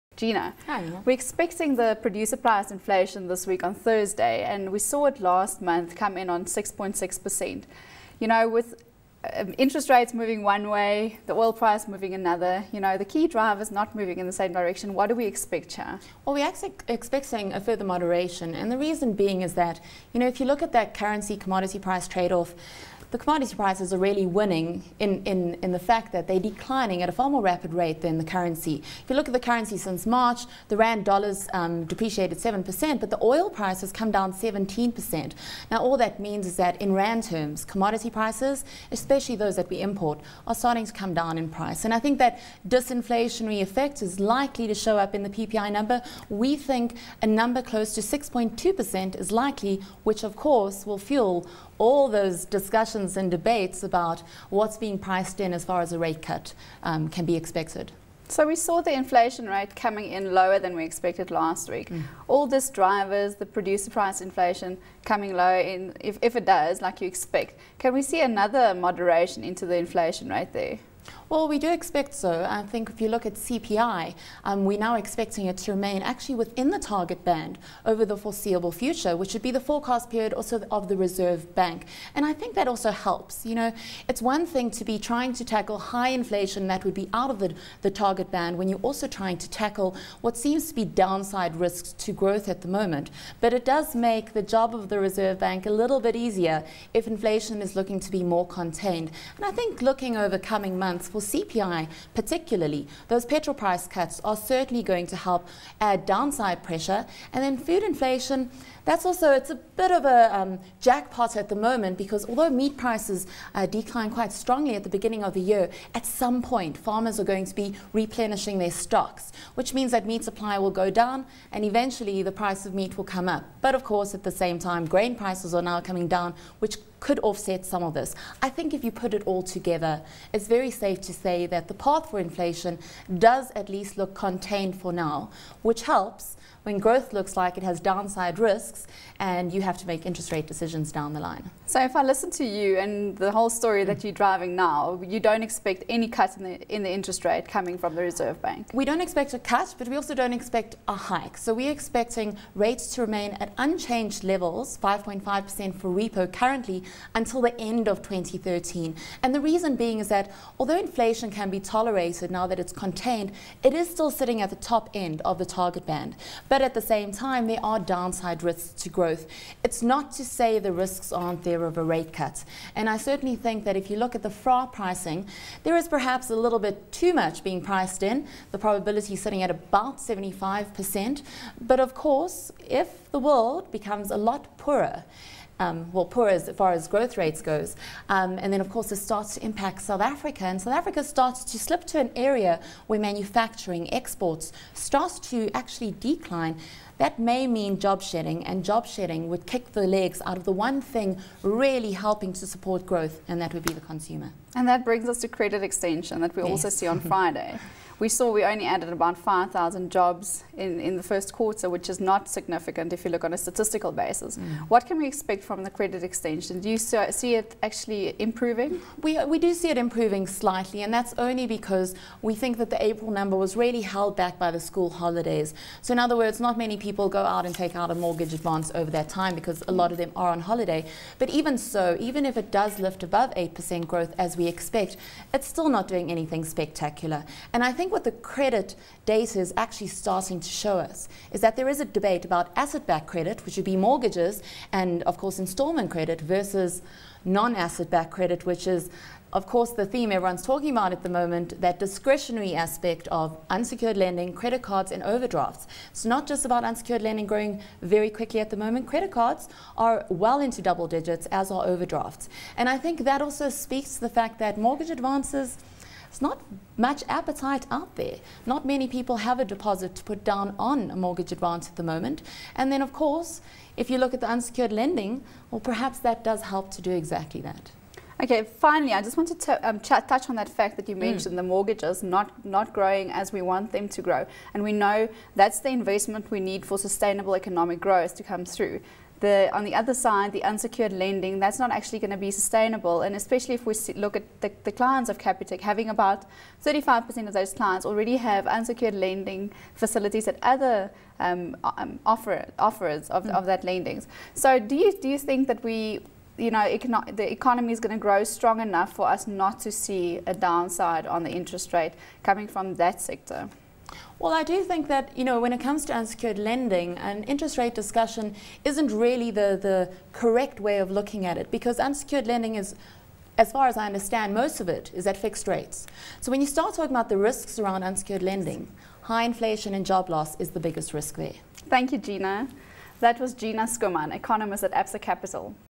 The Economic Week Ahead with Gina Schoeman
The Producer Price Index and SA Trade data is expected to be released this week, with SARB's leading indicator also expected out tomorrow. For a look at this and more, ABN's Mia Kruger speaks with Gina Schoeman, Economist at ABSA Capital.